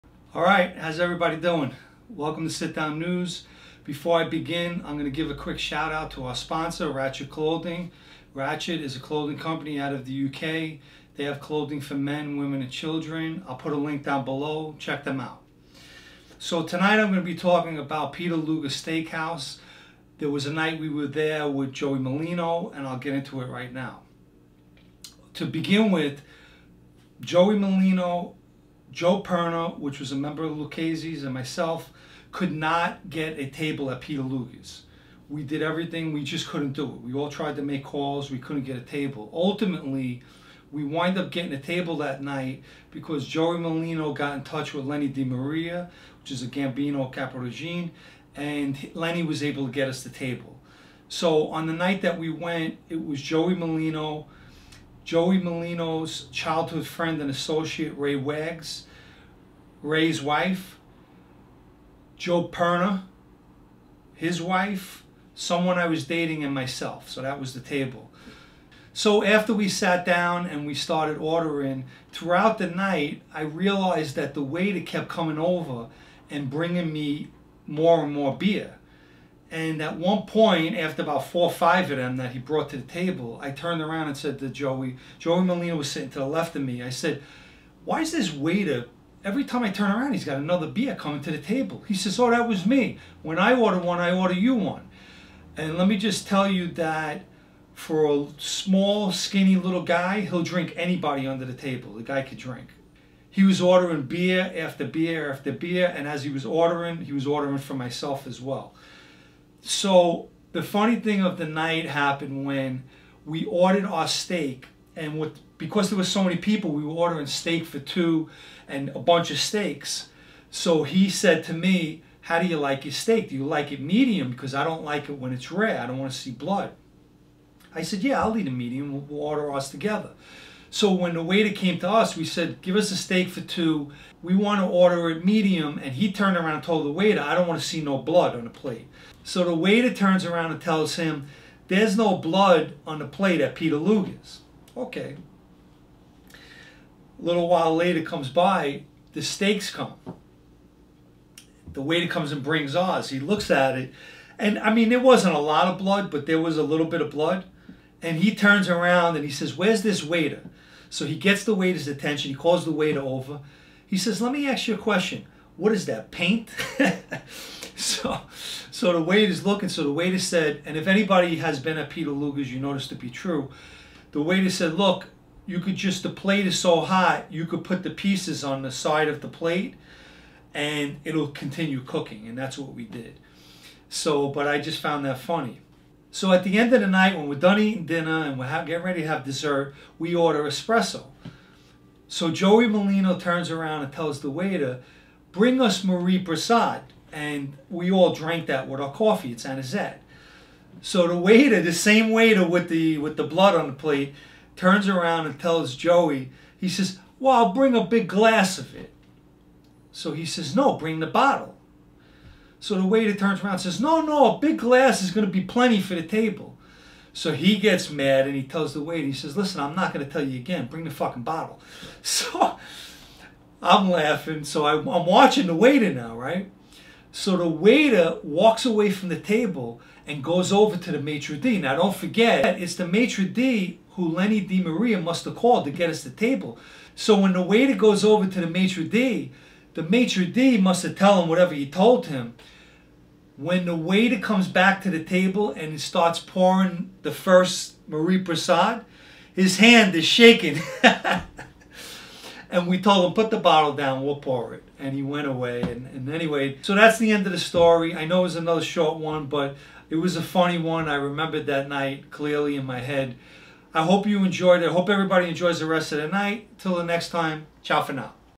Peter Luger Steak House With Joey Merlino
Remembering at night out at Peter Luger Steak House with, Joey Merlino. There was an incident with the waiter over a steak that was too rare for Merlino's liking. Also, the same waiter had trouble pouring our Anisette with a shaking hand.